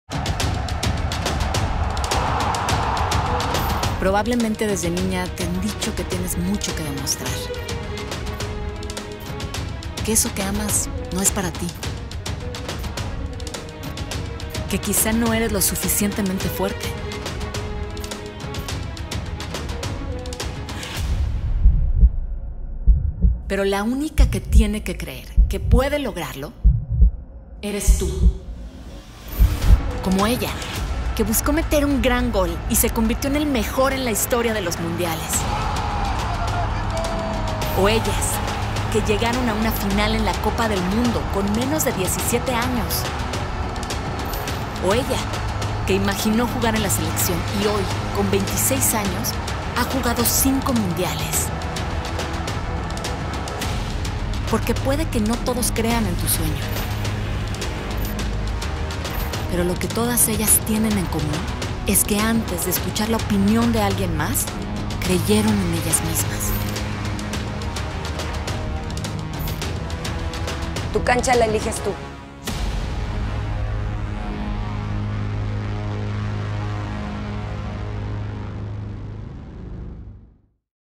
¡EsNuestroMomento! ¡TuCanchaLaEligesTú! | Selección Nacional de México Femenil
Tal vez te han dicho desde niña que no eres lo suficientemente fuerte para lograrlo… Hoy te queremos decir: ¡LO ERES! ¡CREE EN TI MISMA! 💪🏻 🙆🏻‍♀️ 🇲🇽
¡Vayan a seguir nuestras nuevas cuentas de Selecciones Femeniles en Twitter e Instagram! @miseleccionfem 💚